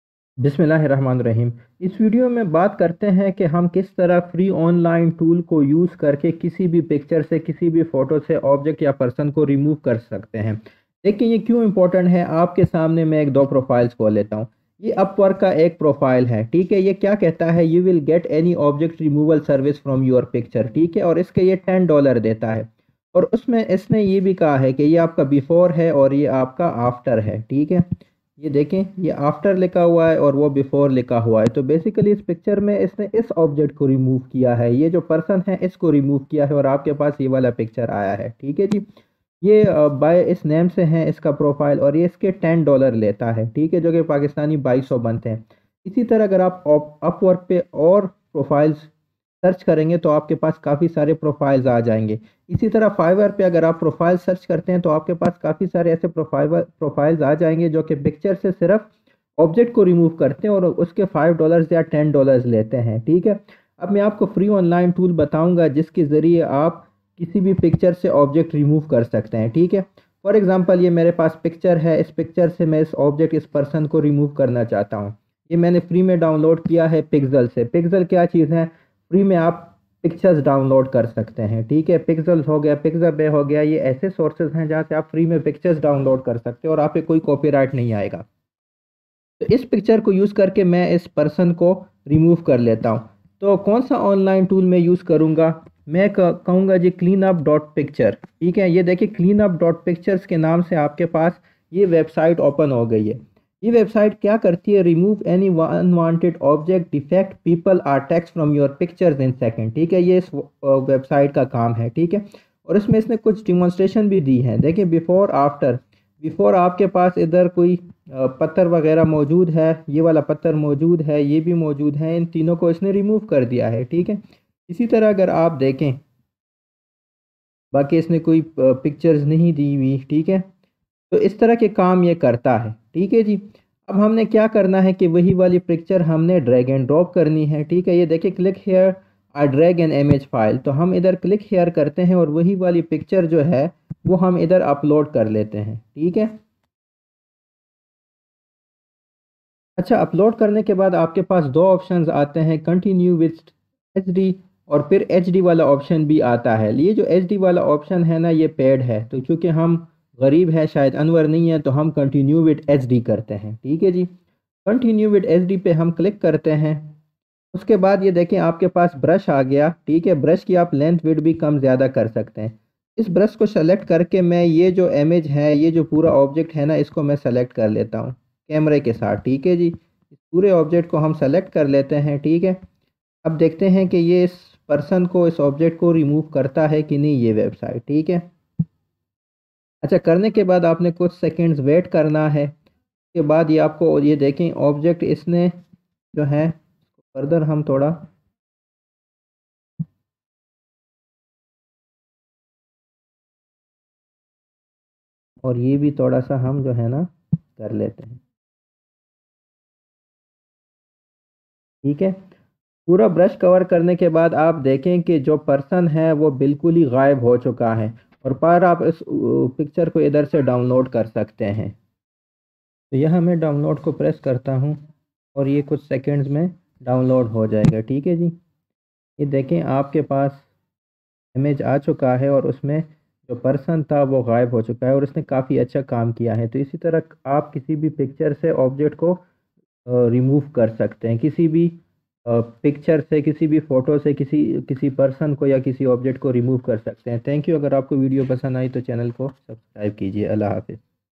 How to earn money removing objects from pictures using free online tool
In this video, we will learn how to remove objects from pictures using free online tool
How to remove objects/ Persons from photos using online tool and earn money
Remove object using AI tool